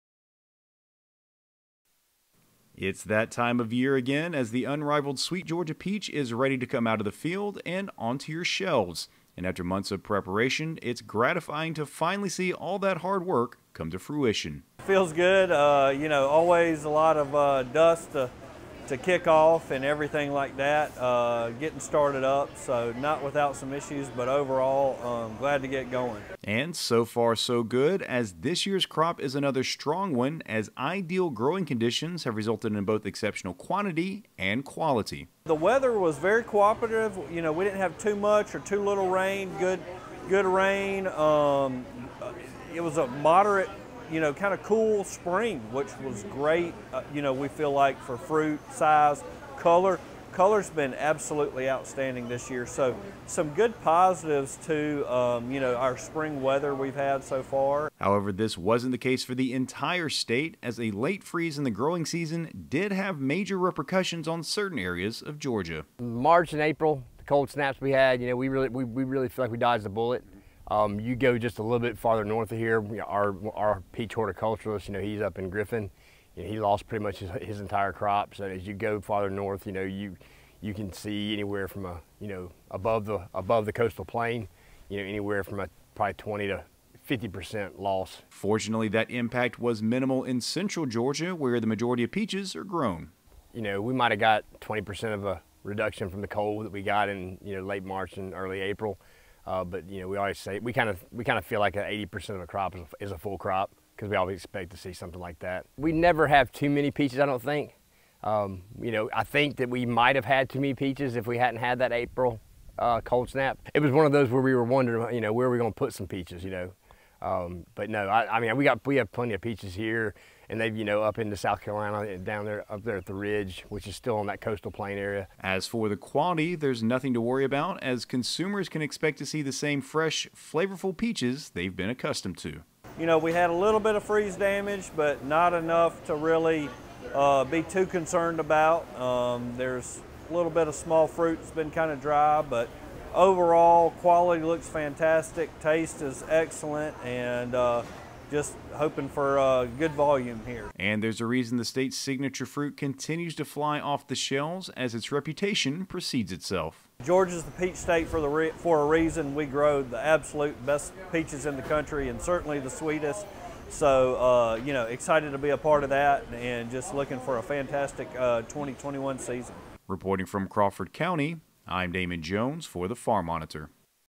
Despite a Late Freeze, Peach Season in Georgia Looking Good
Here in Georgia, the beginning of summer is synonymous with delicious, sweet peaches. Fortunately for consumers, there will be a plentiful supply despite a late freeze. Damon Jones takes us out to Crawford County to check in on this year's harvest.